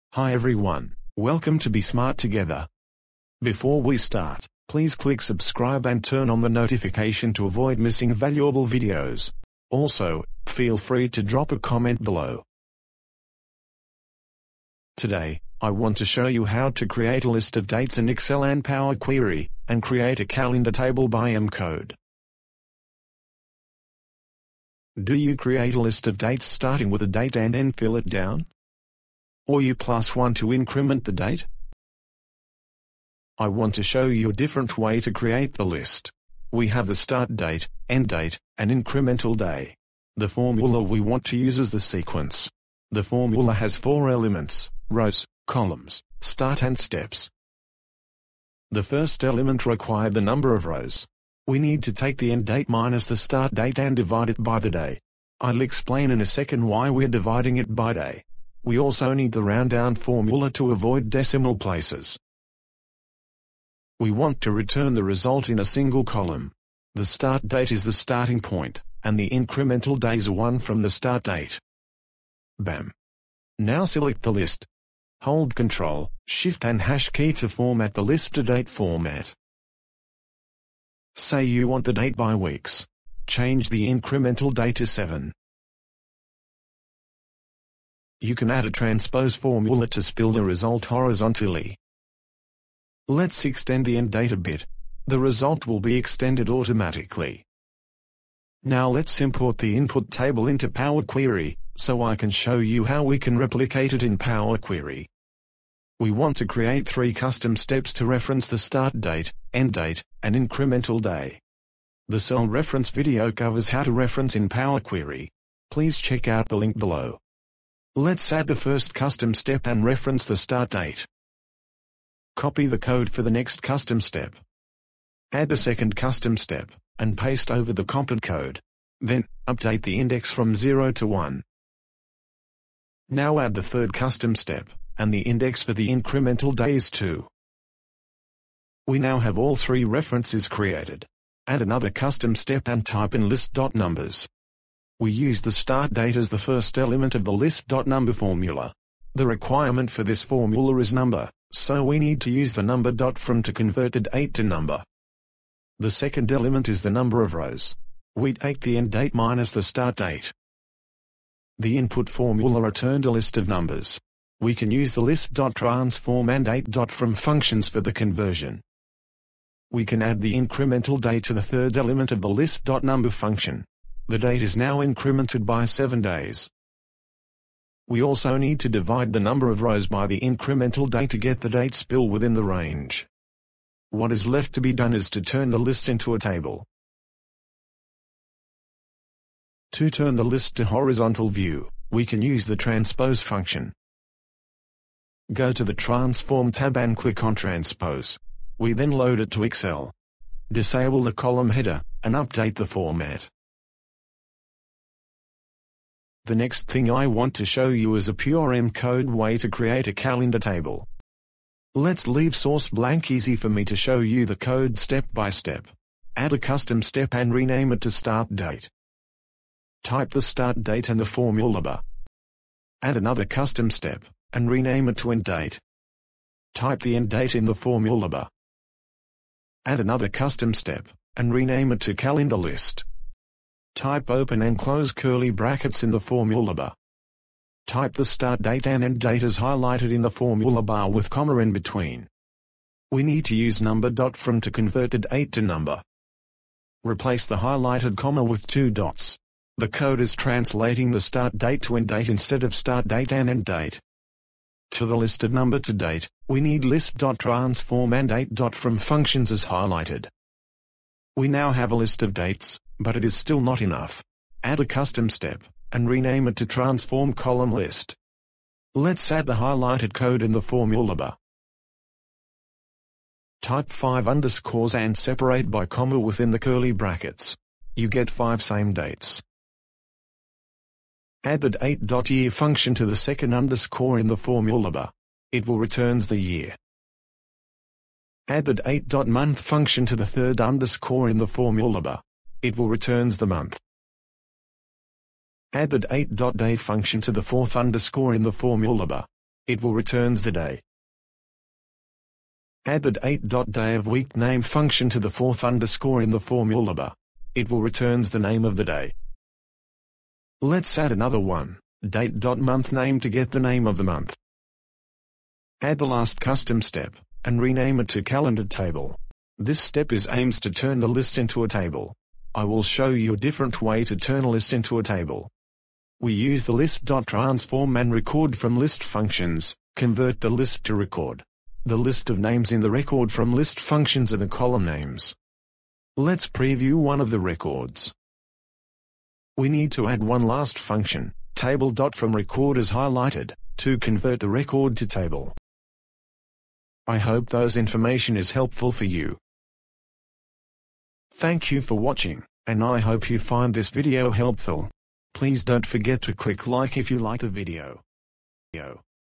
Power Query (Excel) + M Code | Create CALENDAR with SEQUENCE, List.Numbers, & M code Calendar Table
This video will show you how to create an auto spill calendar in Excel and Power Query.

Also, the video covers a pure M code way to build a calendar table.

Topic:
0:00 - Intro
0:40 - Create a list of dates with SEQUENCE
1:46 - Create a list of dates with List.Number
3:26 - Create a Calendar table with M Code
5:46 - Wrap up

Workbook:
N/A